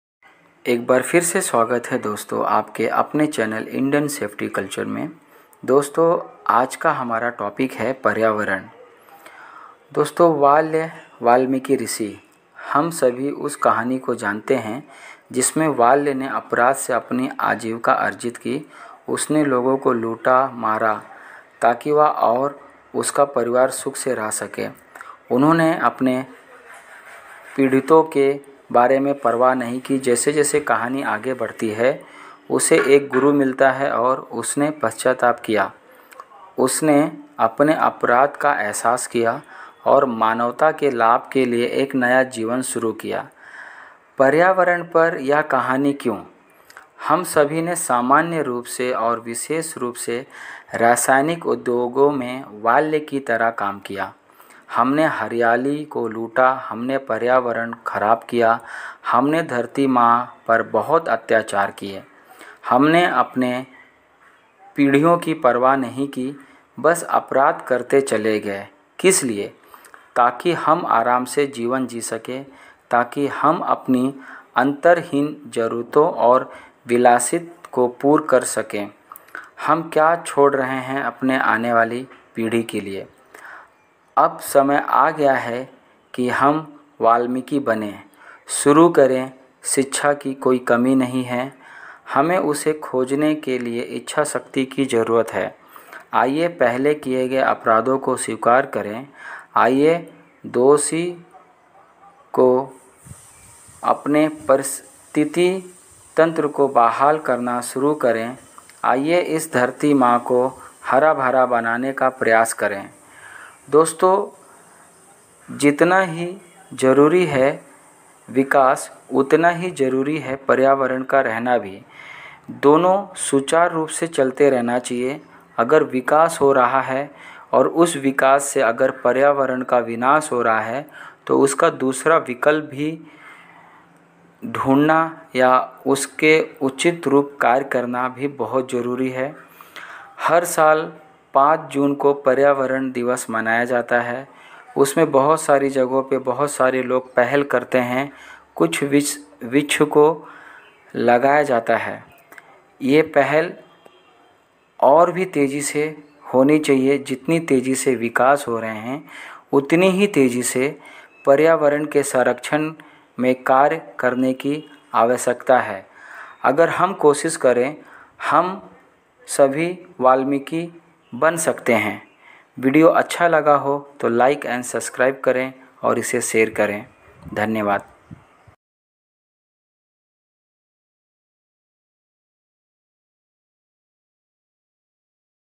विश्व पर्यावरण ### World Environment
World Environment

वाल्याचा वाल्मिकी ऋषि

We all know the story in which Valya earned his livelihood from crime. He looted, he killed so that he and his family could live happily. He did not bother about his victims. As the story goes, he found a Guru, and he repented. He realised his  crime and started a new life for the benefit of humanity.

Why this story on World environment Day?

All of us in general and particularly those in chemical industry have acted like Valya. We have looted the greenery, we have spoiled the environment, we have raped the mother earth. We haven't bothered about our victims, just went on commuting crime For what? So that we live in comfortable life. So that we fulfill our endless needs and luxuries. What are we leaving behind.

It is high time we start becoming Valmiki. There is no shortage of a teacher. We need a will to find him.

Let's first accept the committed crimes, let's be guilty. Let us start Restoring our ecosystem. Let's strive to make this mother earth greener.

We also can become a Valmiki.
पर्यावरण

वालया वालमी ऋषि

हम सभी उस कहानी को जानते हैं जिसमें वाल्या ने अपराध से अपनी आजीविका अर्जित की। उसने लूटा, मार डाला ताकि वह और उसका परिवार सुख से रह सके। उन्होंने अपने पीड़ितों के बारे में परवाह नहीं की। जैसे-जैसे कहानी आगे बढ़ती है, उसे एक गुरु मिला, और उसने पश्चाताप किया। उसने अपने अपराध का एहसास किया और मानवता के लाभ के लिए एक नया जीवन शुरू किया।

पर्यावरण पर यह कहानी क्यों?
हम सभी ने सामान्य रूप से और विशेष रूप से रासायनिक उद्योग में वाल्या की तरह काम किया है। हमने हरियाली को लूटा है, हमने पर्यावरण को खराब किया है, हमने धरती मां का बलात्कार किया है। हमने अपने पीड़ितों की परवाह नहीं की, बस अपराध करते चले गए किसलिए? ताकि हम आराम से जीवन जी सकें। ताकि हम अपनी अंतहीन जरूरतों और विलासिता को पूरा कर सकें। हम क्या छोड़ रहे हैं।

अब समय आ गया है कि हम वाल्मीकि बनना शुरू करें। शिक्षक की कोई कमी नहीं है। हमें उसे खोजने के लिए इच्छाशक्ति की जरूरत है।

आइए पहले किए गए अपराधों को स्वीकार करें, आइए दोषी हों। आइए हम अपने पारिस्थितिकी तंत्र को बहाल करना शुरू करें। आइए इस धरती मां को हरा-भरा बनाने का प्रयास करें।

हम वाल्मीकि भी बन सकते हैं।